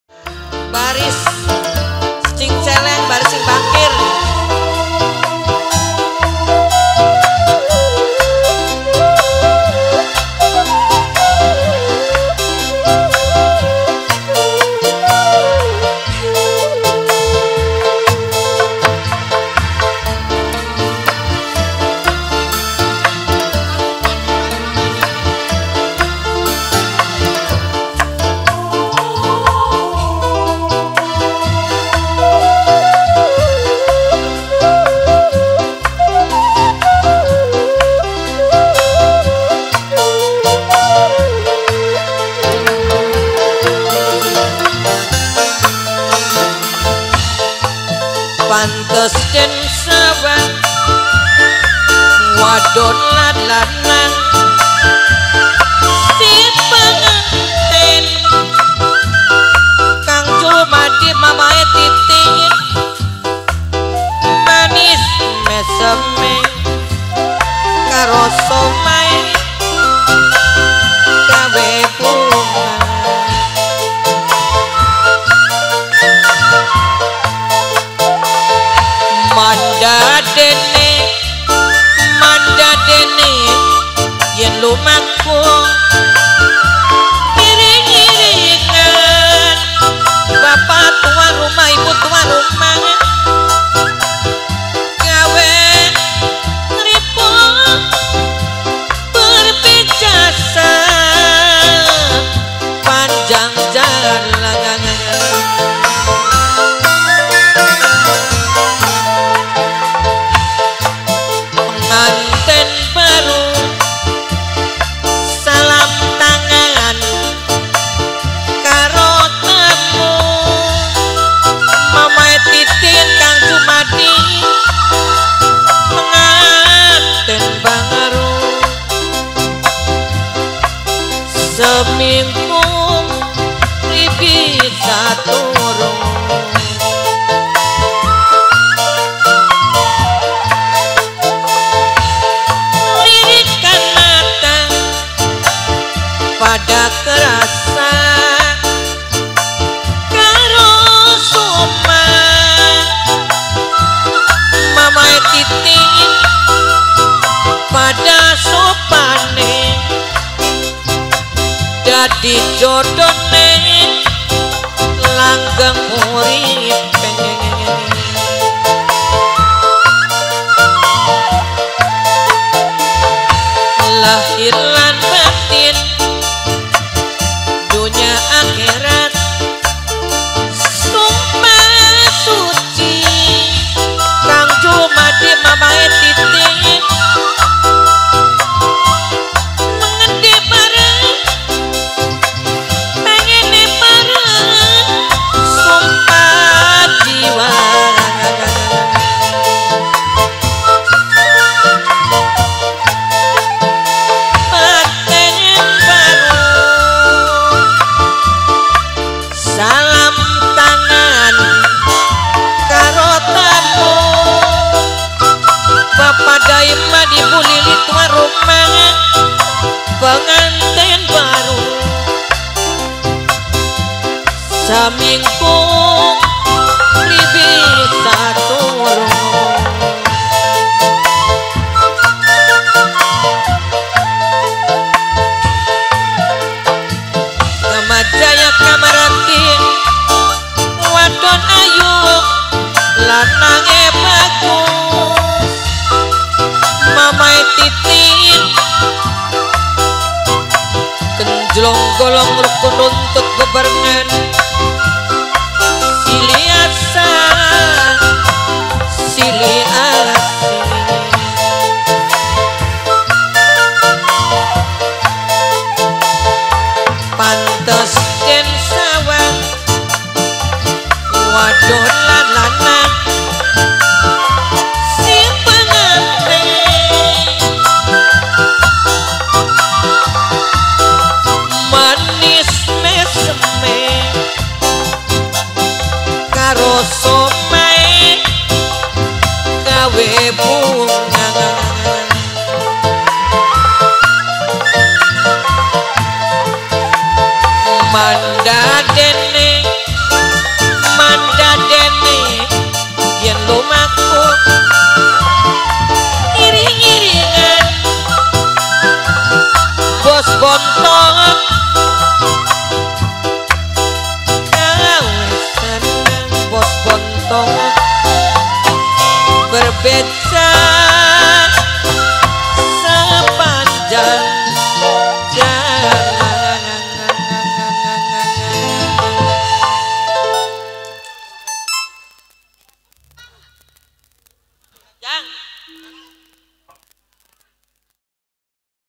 ▶️TEMOAN PENGANTEN BARU, PESTA HAJAT.BPK,DAIMAN / IBU JULILI (LILI) DESA RAMBATAN KULON, 05/10/2022
Dokumentasi Rabu,05 Oktober 2022
Rasulan: DEISYA DAILI Pemangku Hajat: Bpk.Daiman / Ibu Julili (Lili)
Desa Rambatan Kulon (Bangkir) Blok Kangkung / Pom Bensin
Kec.Lohbener Kab.Indramayu

Alamat: Desa Panyindangan Wetan Blok A (Buyut Pekaulan)